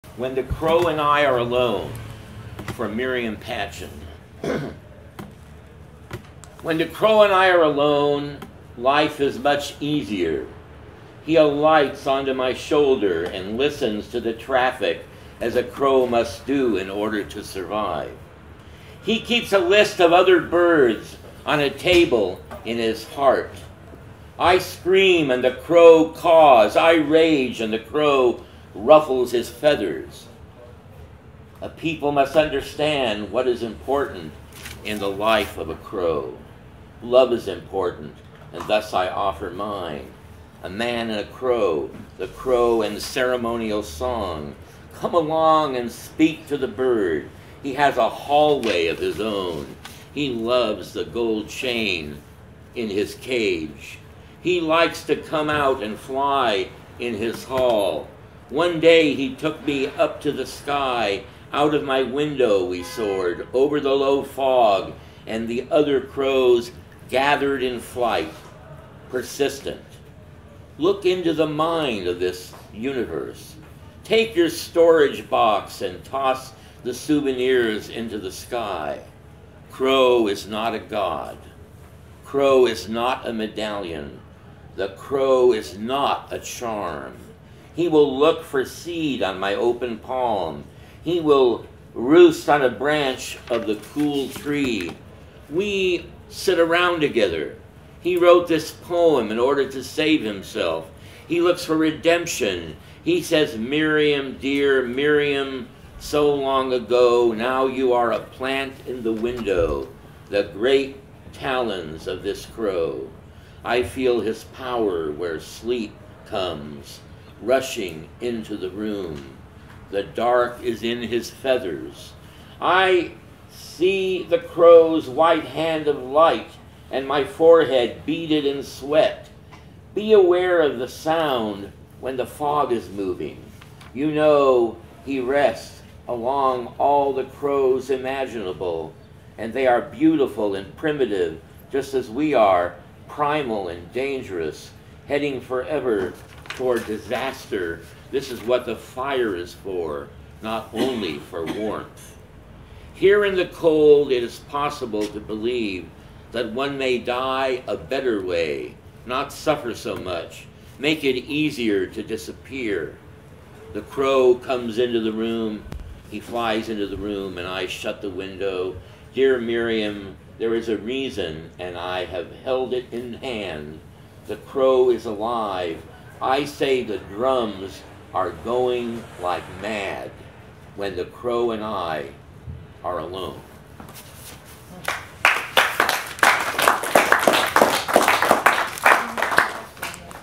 Neeli Cherkovski at Adobe Books --- May 24, 2013
At a commemoration reading for Adobe Books on 16th Street in San Francisco, Neeli Cherkovski shares his poem, When The Crow And I Are Alone (For Miriam Patchen), on May 24th, 2013. Adobe Books has moved to 3130 24th Street.